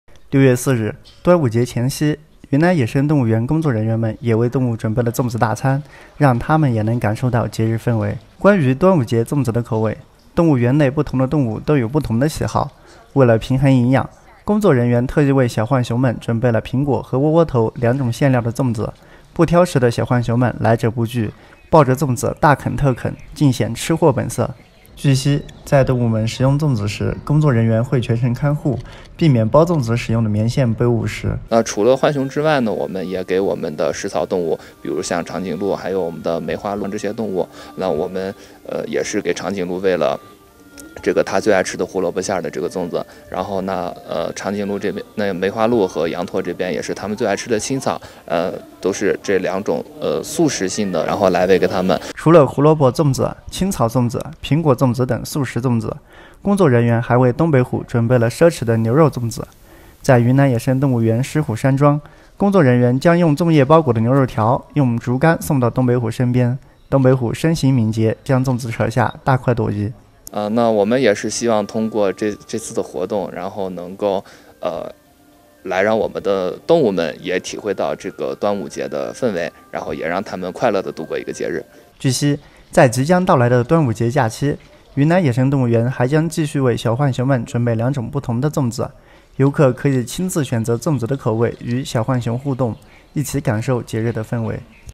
动物也要迎端午！云南野生动物园给动物“定制”粽子大餐
6月4日，端午节前夕，云南野生动物园工作人员们为动物准备了粽子大餐，让它们也能感受到节日氛围。关于端午节粽子的口味，动物园内不同的动物都有不同的喜好。为了平衡营养，工作人员特意为小浣熊们准备了苹果和窝窝头两种馅料的粽子。不挑食的小浣熊们来者不拒，抱着粽子大啃特啃，尽显“吃货本色”。据悉，在动物们食用粽子时工作人员会全程看护，避免包粽子使用的棉线被误食。（刀志楠）